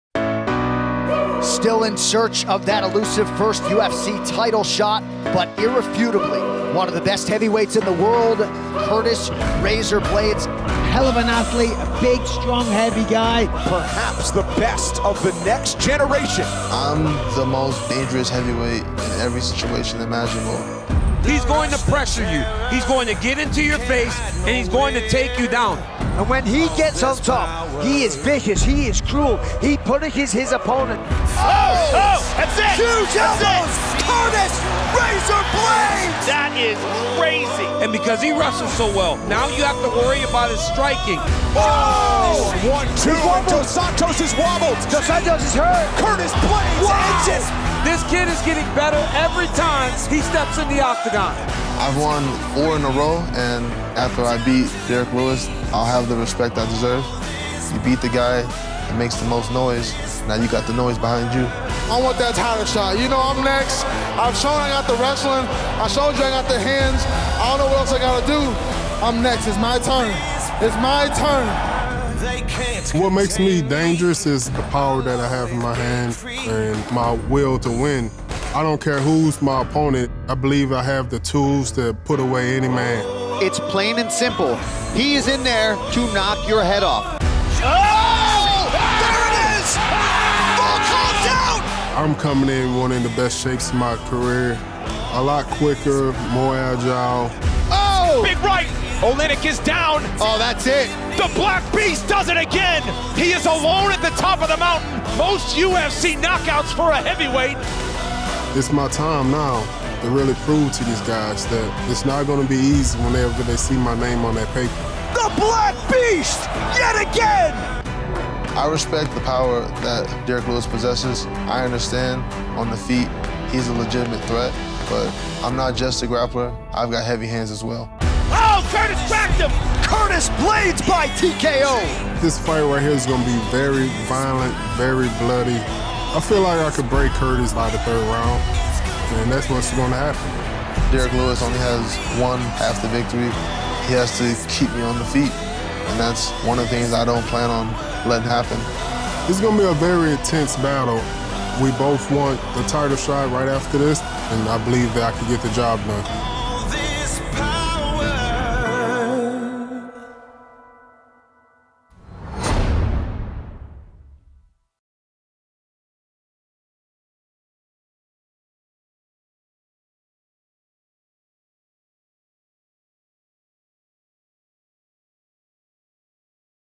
UFC Vegas 19: Blaydes vs Lewis - It's My Time Now | Fight Preview
Preview the heavyweight matchup headlining UFC Vegas 19 this weekend between top five contenders Curtis Blaydes and Derrick Lewis. Saturday's main card starts at 10pm ET / 7pm PT.